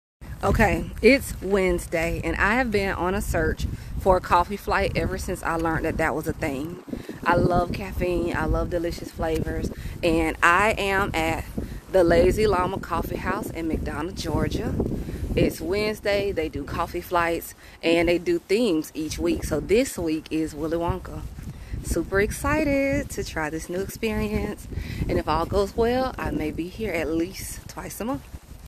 The Lazy Llama Coffee Shop in McDonough, GA has delicious coffee flights
First time experiencing a coffeeflight and I am in love!!! The vibe at @thelazyllamallc is so nice and I plan to visit at least 2xs a month on Wednesdays for the new flavors!!! Great customer service!

Videos to come of the full experience but highly recommend when you are in the McDonough area